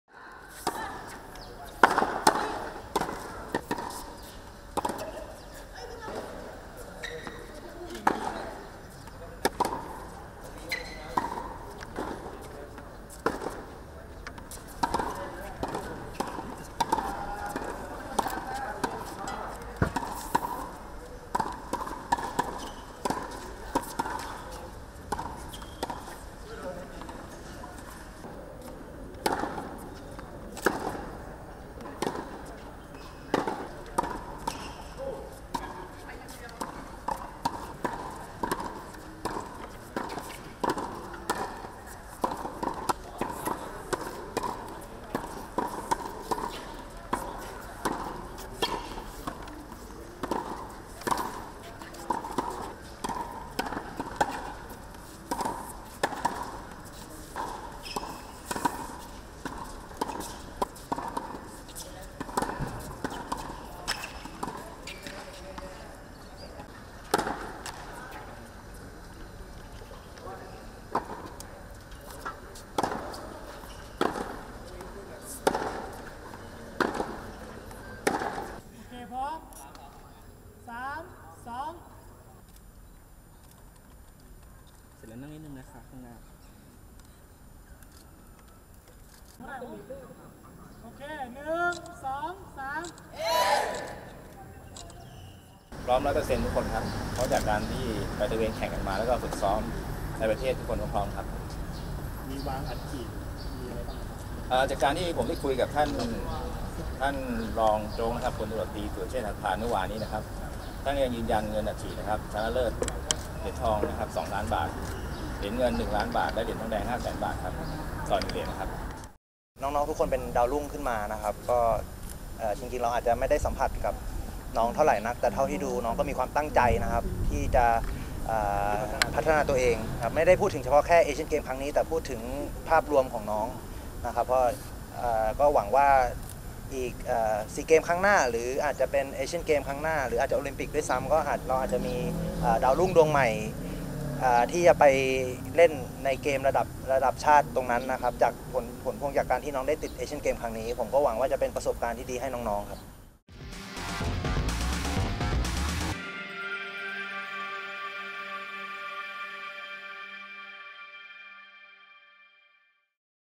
เทนนิสวางอัดฉีดคว้าทองรับ 2 ล้าน สร้างขวัญกำลังใจลุยศึกเอเชี่ยนเกมส์
ความเคลื่อนไหวนักเทนนิสทีมชาติไทยชุดเอเชี่ยนเกมส์ 2018 นำโดย “ลัก” ลักษิกา คำขำ เดี่ยวมือหนึ่งสาวไทย เจ้าของเหรียญทองหญิงคู่ และ 1 เหรียญเงินหญิงเดี่ยวเอเชี่ยนเกมส์ 2014 ที่ประเทศเกาหลีใต้ ได้นำทีมลงฝึกซ้อมเตรียมความพร้อม ที่ศูนย์พัฒนากีฬาเทนนิสแห่งชาติ เมืองทองธานี เมื่อช่วงเช้าที่ผ่านมา

ส่วนนักเทนนิส ประกอบด้วย ประเภทหญิง “อีฟ” ณิชาต์ เลิศพิทักษ์สินชัย รวมถึงนักหวดน้องใหม่ “ไหม” มนัญชญา สว่างแก้ว, “แนต” พัณณิน โควาพิทักษ์เทศ, ประเภทชาย “เน็ต” พลภูมิ โควาพิทักษ์เทศ, “บูม” กษิดิศ สำเร็จ “จูเนียร์” วิชยา ตรงเจริญชัยกุล, “ต้น” สนฉัตร รติวัฒน์, “ต้อง” สรรค์ชัย รติวัฒน์ นักหวดคู่แฝดจอมเก๋า, “แมน” ณัฐนนท์ กัจฉปานันท์, “เอิร์ธ” เพียงธาร ผลิพืช และ “แต้ว” ทรรศพร นาคหล่อ

โดยมี ร้อยตำรวจเอก วรพล ทองคำชู ผู้ช่วยผู้จัดการทีม เดินทางมาร่วมชมการฝึกซ้อม และเผยว่าเป้าหมายในตอนนี้คงยังฟันธงไม่ได้ เพราะจากรายชื่อที่ออกมา แต่ละประเทศได้ส่งมือดีเข้าร่วมการแข่งขันเพื่อเก็บเหรียญรางวัล ไม่ว่าจะเป็น อินเดีย, จีน, ญี่ปุ่น, เกาหลีใต้ รวมถึงอุซเบกิสถานแต่เราจะทำให้ดีที่สุด ถึงตรงนี้นักกีฬาทุกคนร่างกายพร้อมร้อยเปอร์เซ็นต์ หลังจากการฝึกซ้อมในประเทศ รวมถึงตระเวรแข่งขันรายการต่างๆ อย่างไรก็ตาม จากการที่ได้พูดคุย พลตำรวจตรีสุรเชษฐ์ หักพาล รองผู้บัญชาการตำรวจท่องเที่ยว และอุปนายกสมาคมกีฬาเทนนิสแห่งประเทศไทยฯ ยืนยันเงินอัดฉีด เหรียญทอง เหรียญละ 2 ล้านบาท เหรียญเงิน เหรียญละ 1 ล้านบาท และ เหรียญทองแดง 5 แสนบาท เพื่อเป็นขวัญและกำลังใจให้แก่นักกีฬา ในการแข่งขันครั้งนี้

ด้าน ลักษิกา เปิดเผยว่าเป้าหมายจริงๆอยากดูไปรอบต่อรอบ ยอมรับว่าลึกๆอยากจะทำให้ได้เท่าครั้งก่อนหรือดีกว่า แต่ก็ไม่อยากกดดันตัวเองมากนัก คงต้องขอดูรอบต่อรอบ และจะทำหน้าที่ให้ดีที่สุด เพื่อความสุขของแฟนเทนนิสชาวไทยทุกคน

ส่วน สนฉัตร และ สรรค์ชัย รติวัฒน์ คู่แฝดจอมเก๋า ก็บอกว่าครั้งนี้ไม่ง่านแน่นอน เพราะมีนักกีฬามือดีเข้าร่วมการแข่งขันด้วยหลายคน โดยเฉพาะประเภทคู่ ซึ่งได้เจอกันในรายการอาชีพบ้าง ทำให้พอจะรู้ฝีมือ ทุกชาติต่างหวังจะเก็บเหรียญรางวัล ดังนั้นเขาต้องส่งผู้ล่นชุดที่ดีที่สุดมาแข่ง และก็ได้พูดถึงนักเทนนิสรุ่นน้องที่ติดทีมชาติชุดนี้ด้วย

สำหรับนักเทนนิสชุดเอเชี่ยนเกมส์ ครั้งที่ 18 นั้น จะออกเดินทางไปยังเมืองปาเล็มบัง ประเทศอินโดนีเซีย ชุดแรก วันที่ 16 สิงหาคม ได้แก่ สนฉัตร รติวัฒน์, สรรค์ชัย รติวัฒน์, วิชยา ตรงเจริญชัยกุล, ณัฐนนท์ กัจฉปานันท์, พลภูมิ โควาพิทักษ์เทศ, กษิดิศ สำเร็จ, ลักษิกา คำขำ

ส่วนชุดที่สอง ออกเดินทางวันที่ 17 สิงหาคม ได้แก่ เพียงธาร ผลิพืช, ณิชาต์ เลิศพิทักษ์สินชัย, ทรรศพร นาคหล่อ, มนัญชญา สว่างแก้ว ขณะที่ชุดสุดท้ายวันที่ 18 สิงหาคม พัณณิน โควาพิทักษ์เทศ พร้อมสตาฟฟ์โค้ช